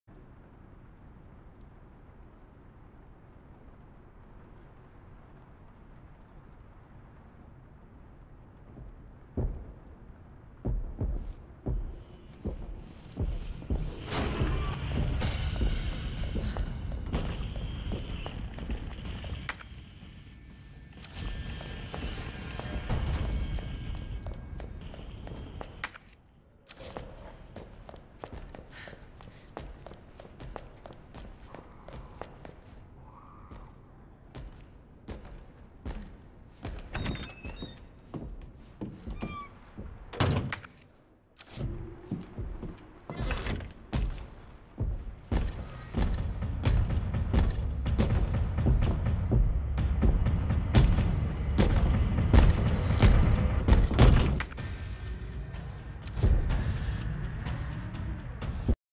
"The clock tower is safe from Mr. X"
So... this was the first time he did this and I got so scared he was gonna come in I almost had a heart attack

"Why do you keep checking the map" because I was paranoid and also my spatial awareness is very poor